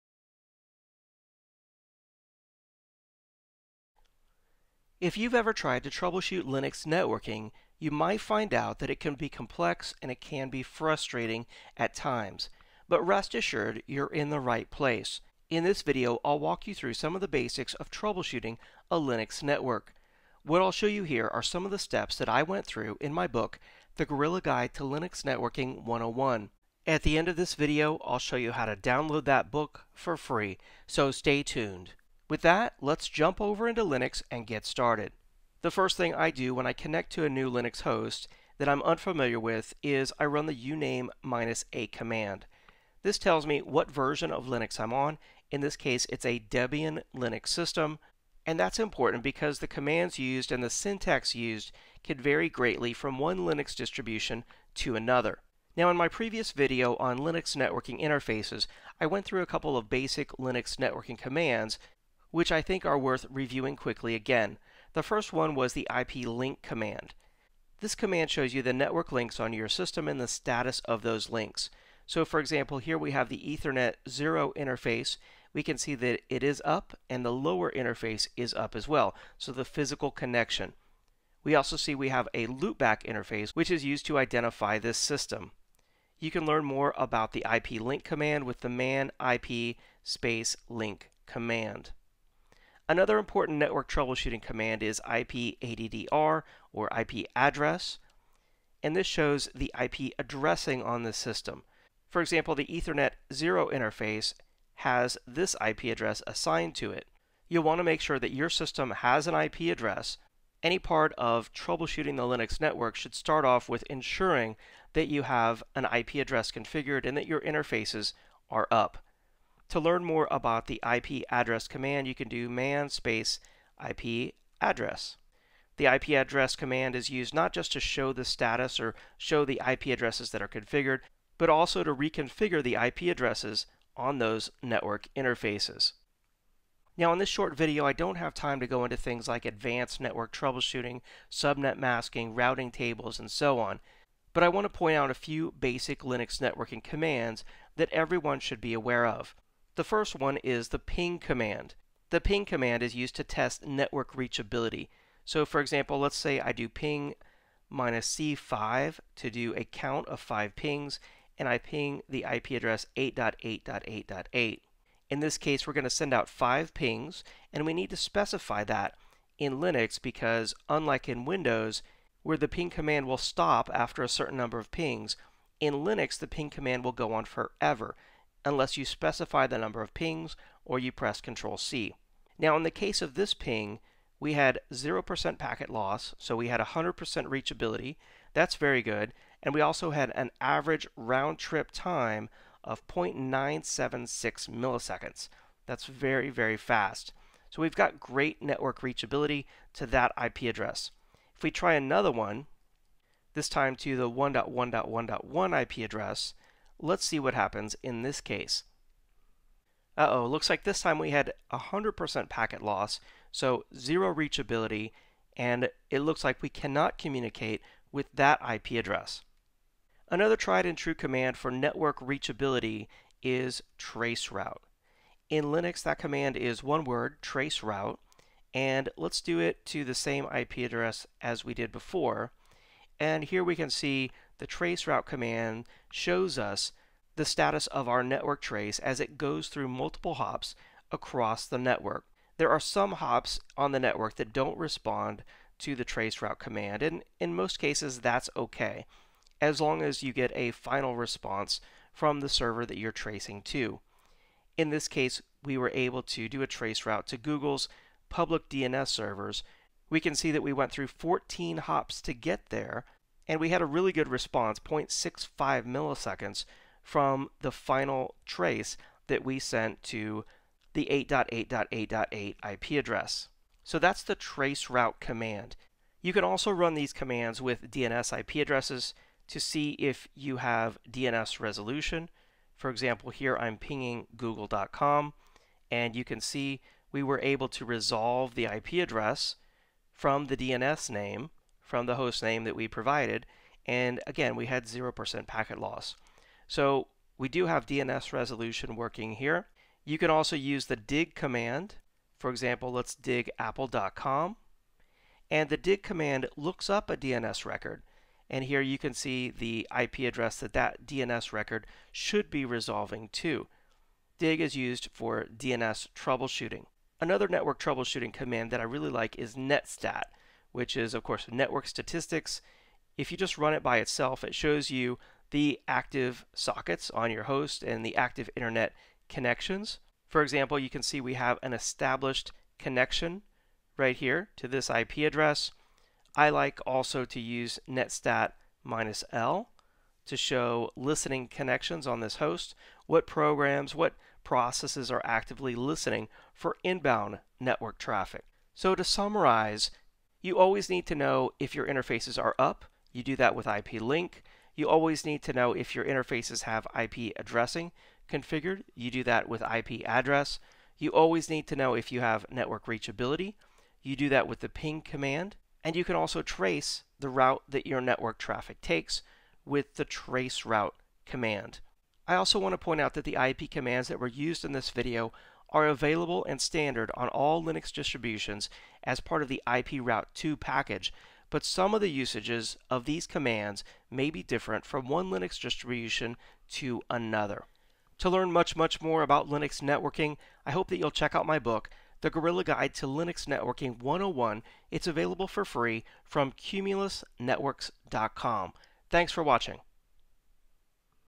Basic Linux Network Troubleshooting
David Davis, author of "The Gorilla Guide to Linux Networking 101", walks through and demos troubling shooting a Linux network.

Watch all of David's Linux 101 videos